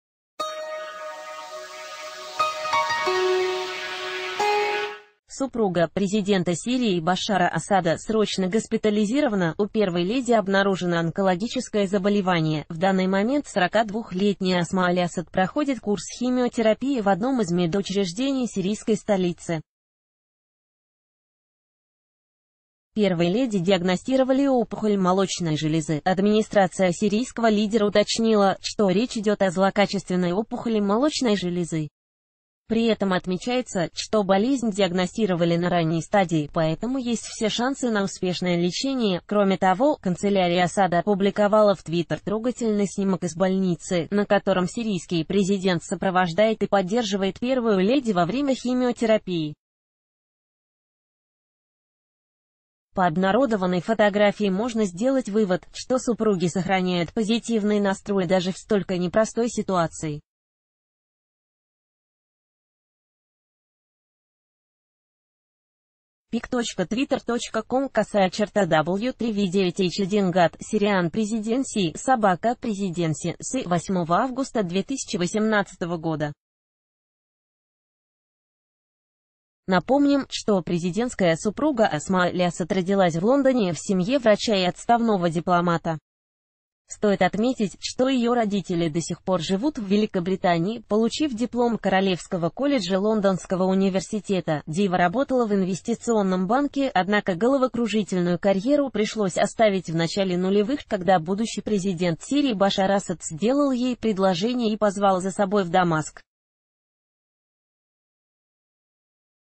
У красавицы-жены Асада нашли рук груди | TVRu
Супруга президента Сирии Башара Асада срочно госпитализирована. У первой леди обнаружено онкологическое заболевание. В данный момент 42-летняя Асма аль-Асад проходит курс химиотерапии в одном из медучреждений сирийской столицы. Первой леди диагностировали опухоль молочной железы.Администрация сирийского лидера уточнила, что речь идет о злокачественной опухоли молочной железы. При этом отмечается, ...